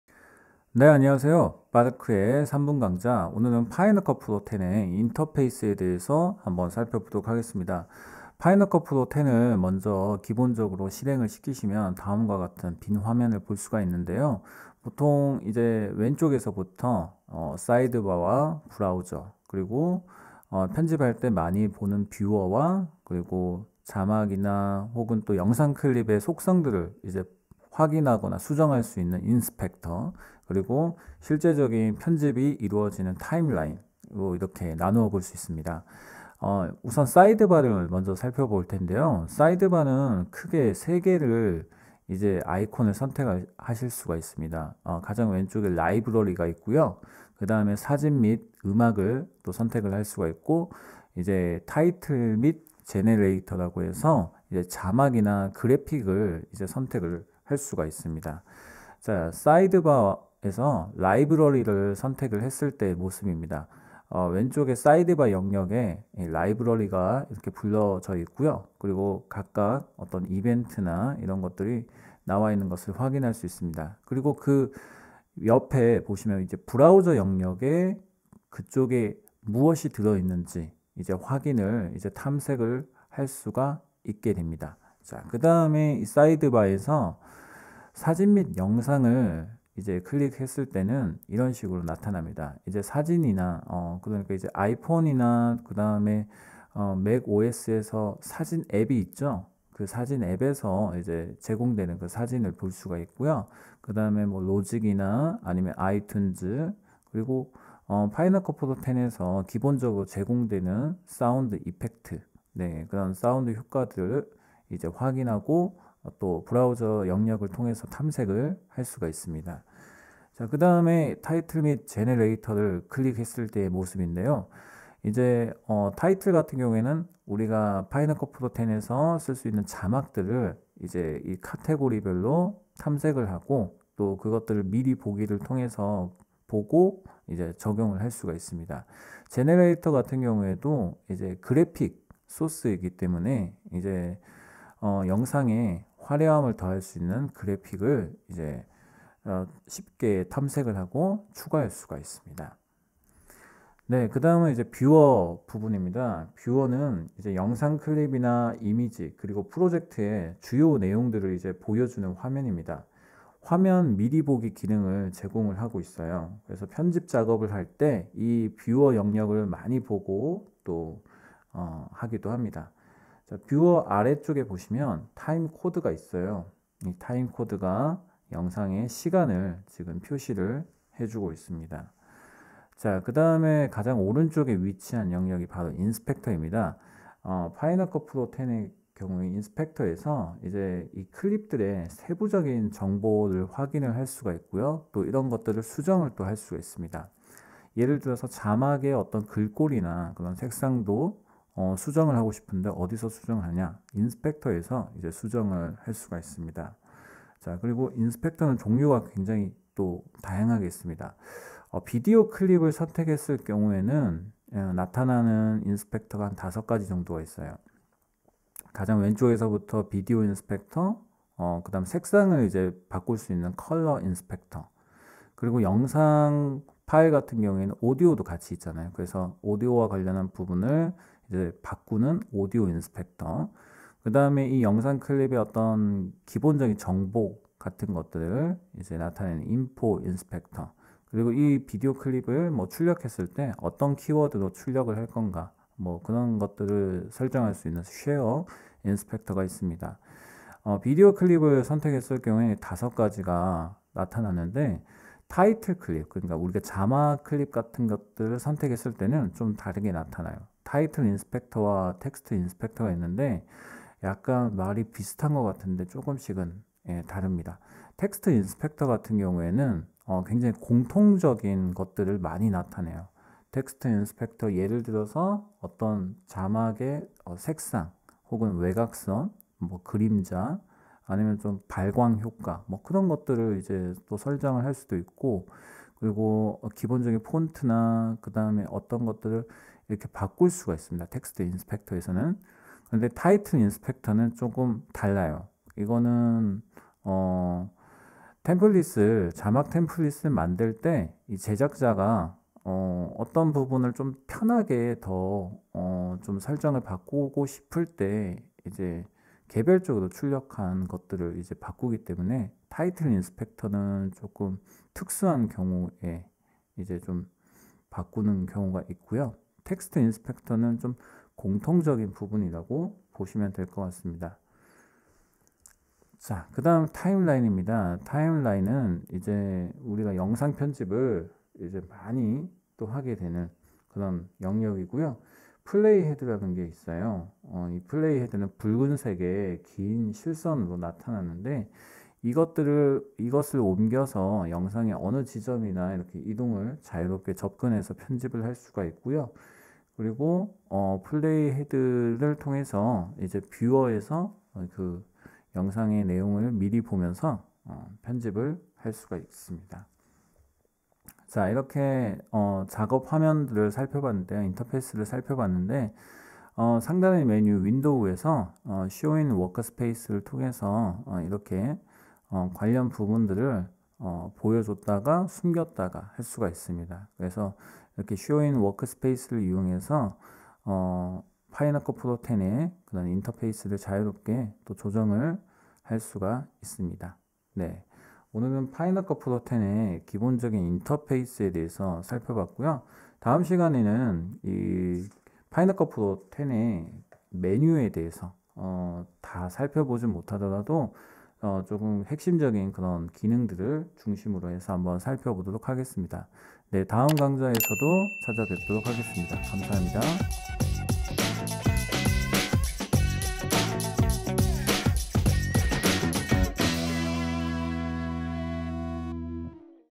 빠르크의 파이널컷프로 3분강좌 38강 파이널컷프로X 인터페이스
안녕하세요 빠르크의 파이널컷프로X 3분강좌 38강으로 오늘은 파이널컷프로X의 인터페이스에 대해 살펴보도록 하겠습니다.

사이드 바, 브라우저, 뷰어, 인스펙터, 타임라인으로 크게 나눠볼 수 있습니다. 각각 어떤 형태와 기능들이 있는지 살펴볼게요.